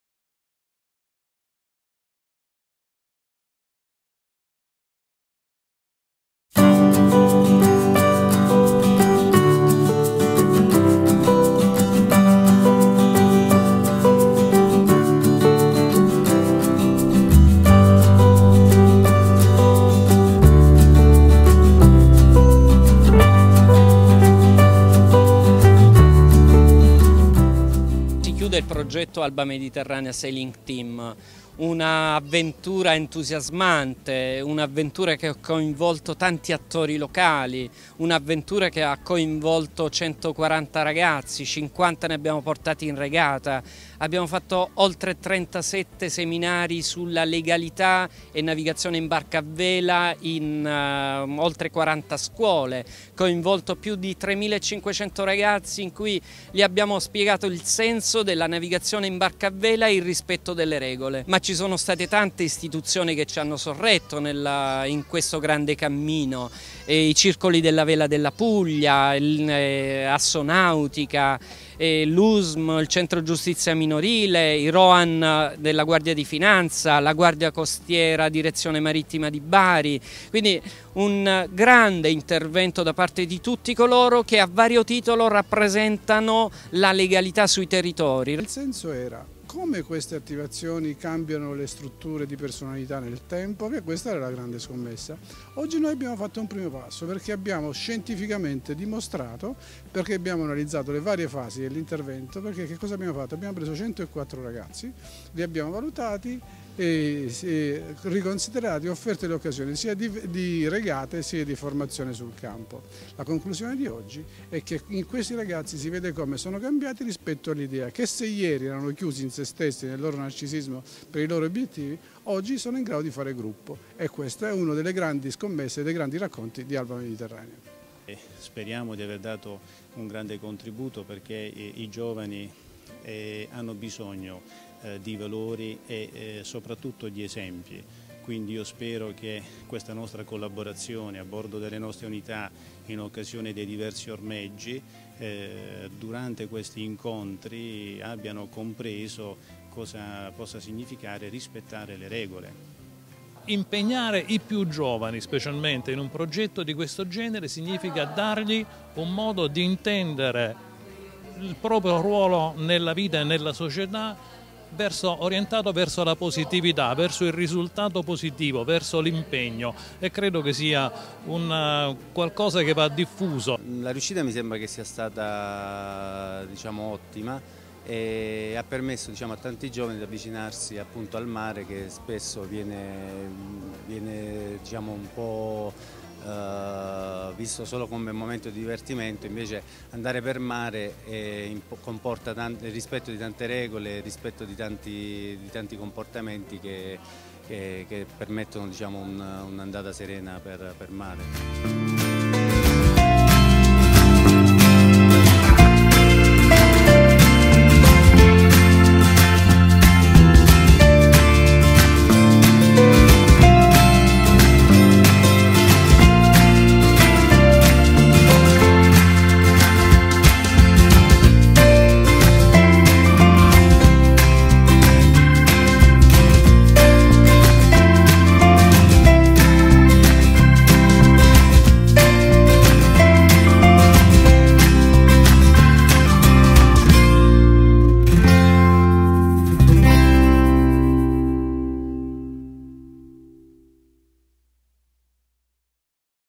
Alba Mediterranea. Bilancio 2017 alle Manifatture Knos
"Navigare oltre le barriere". Il percorso di ALBA MEDITERRANEA - Associazione di Promozione Sociale è fatto di impegno e dedizione, attraverso il lavoro di squadra sulla barca a vela e il cammino per strada. Alle Manifatture Knos si è tenuto l'incontro durante il quale si è presentato il bilancio dei risultati raggiunti da questo progetto straordinario, alla presenza delle scuole e delle autorità marittime coinvolte nelle diverse tappe del percorso. Da anni seguiamo ALBA MEDITERRANEA e i suoi "Messaggeri di Legalità" nel loro bellissimo viaggio, ecco il nostro video racconto.